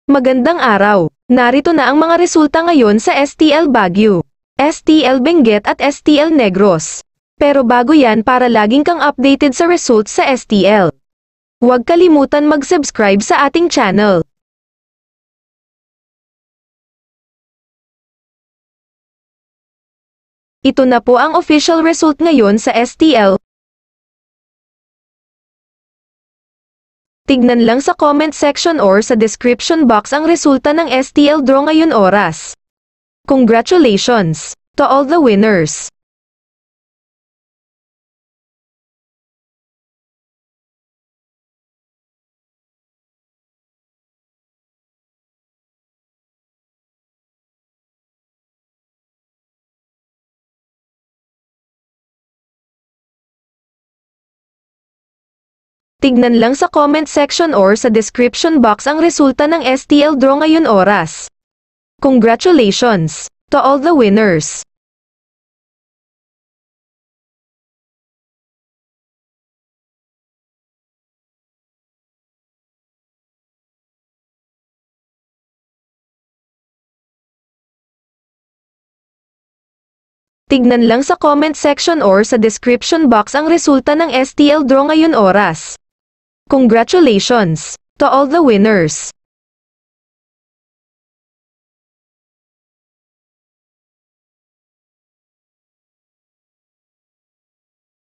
STL BAGUIO, STL BENGUET & STL NEGROS 7PM, LIVE DRAW April 17, 2022 Evening Draw Result Today
07:00PM 3RD  DRAW RESULT ↓↓↓

BAGUIO → 02 - 12
BENGUET→ 03 - 23
NEGROS / BACOLOD → 34 - 10

03:00PM 2nd  DRAW RESULT ↓↓↓

BAGUIO → 07 - 35
BENGUET→ 36 - 19
NEGROS / BACOLOD → 28 - 24

10:30AM 1st  DRAW RESULT ↓↓↓

BAGUIO → 38 - 11
BENGUET→ 21 - 22
NEGROS / BACOLOD → 33 - 08